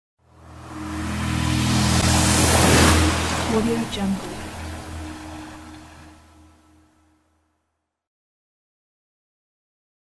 Newspaper Letters Logo Reveal
HD resolution Insert your logo. Change color you want. Pre-render trapcode particles as flying letters. Pre-render optical flares. So no plugin needed. Audio not included but you can find it Please rate if you like it.